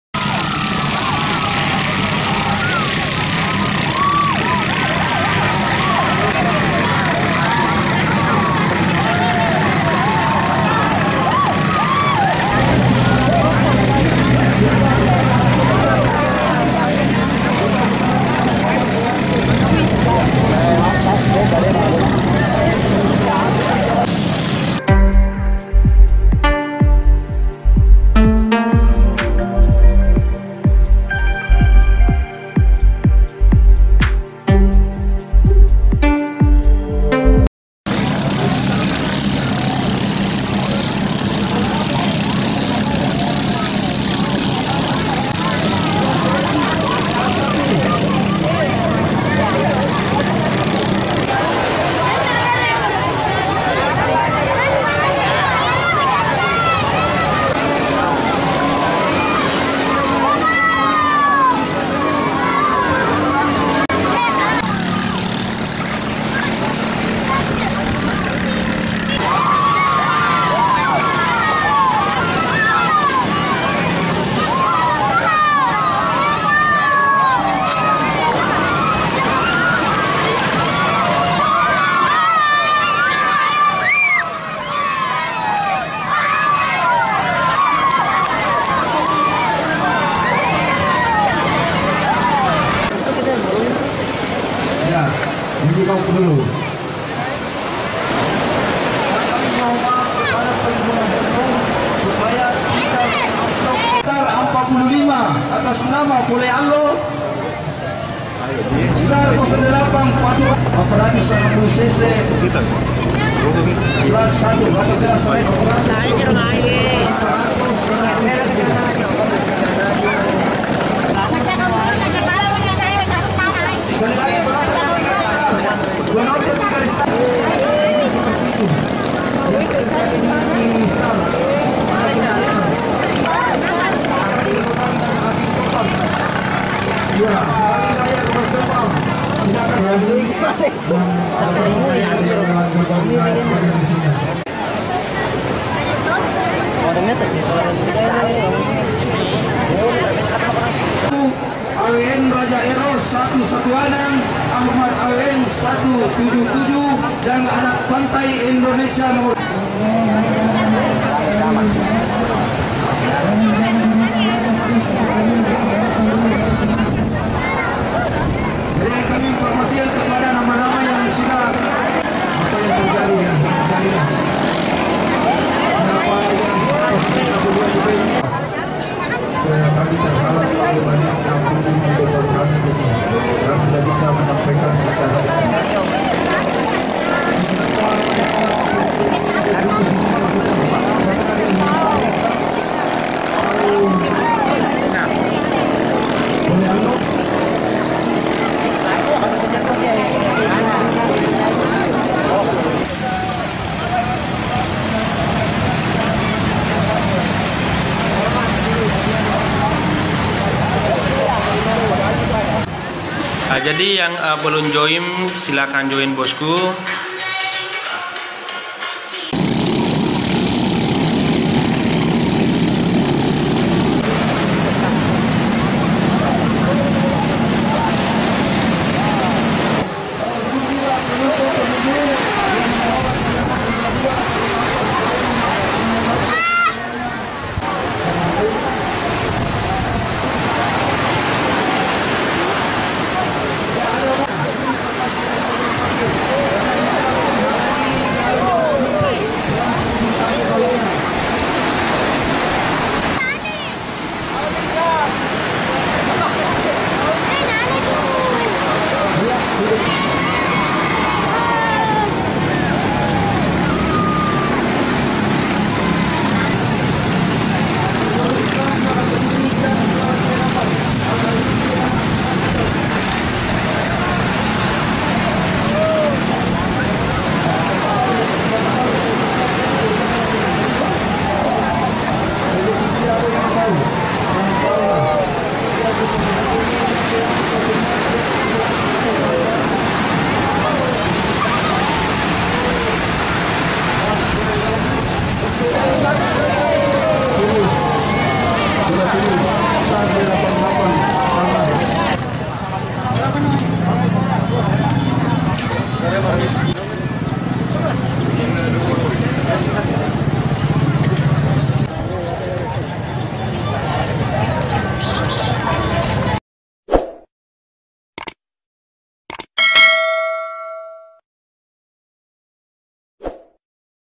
BALAP PERAHU NELAYAN { kecepatan Super }
kecepatan super dipertontonkan sejumlah nelayan yang ikut balap Perahu